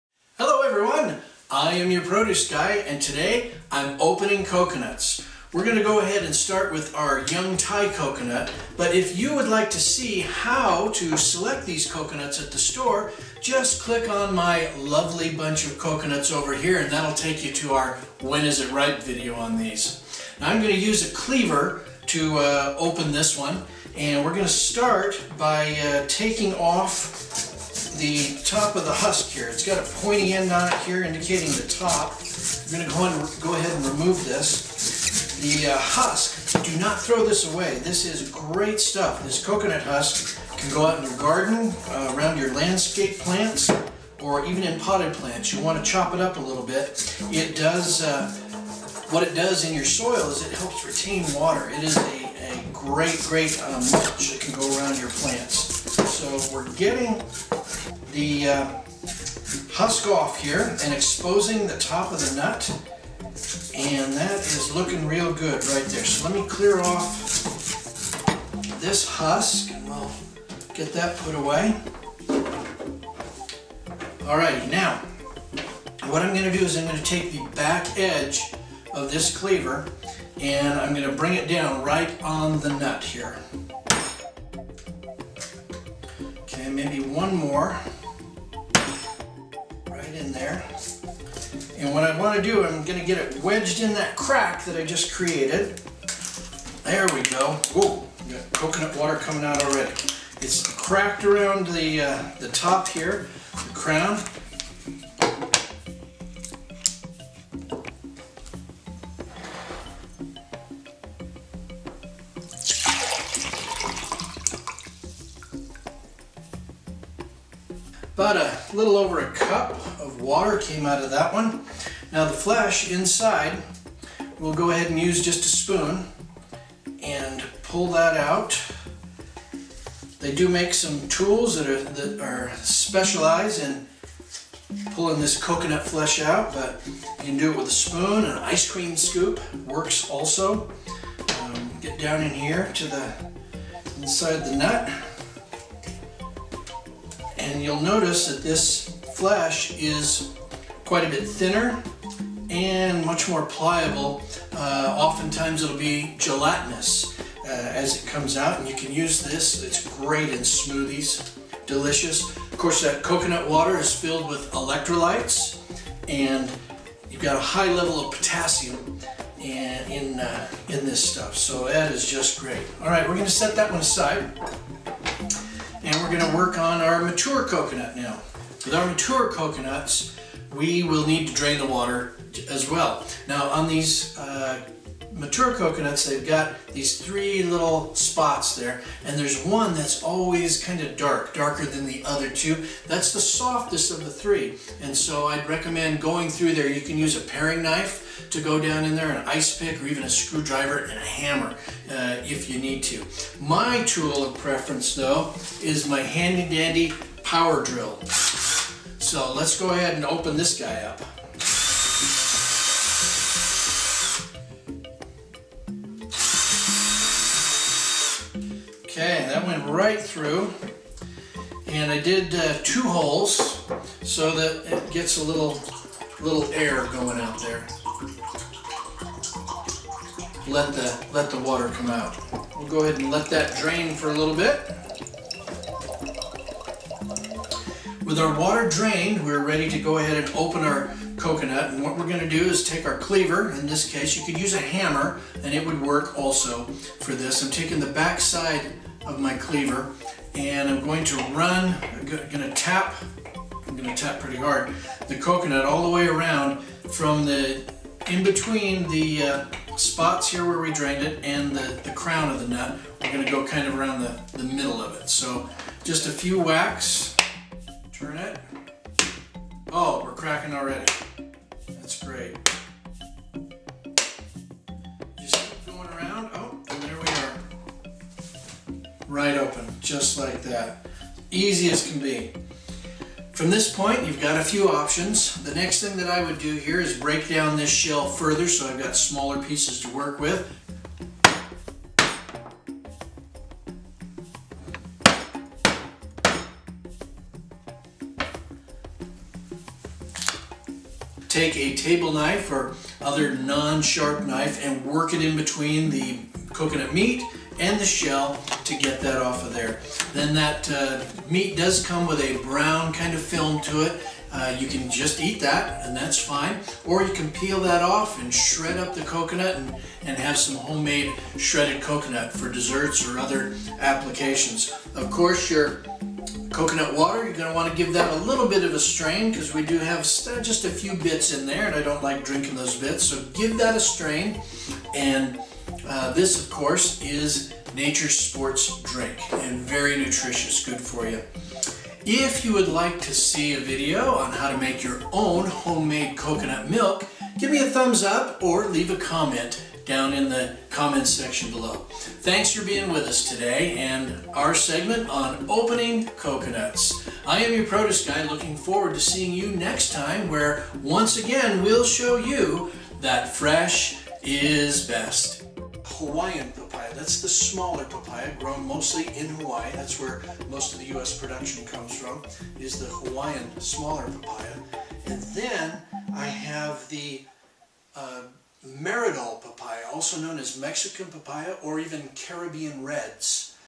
How To Open A Coconut
YourProduceGuy shows you how to open a coconut. Both the young coconut and mature hard coconut. These are so delicious and nutritious. They are natures sports drink, full of electrolytes, potassium and other stuff that is great for you. And that's just the juice. Coconuts are also loaded with healthy fats that your body needs to work properly. Come and see how to get this coconut deliciousness fresh rather than out of a can.

KEEP UP WITH YOURPRODUCEGUY!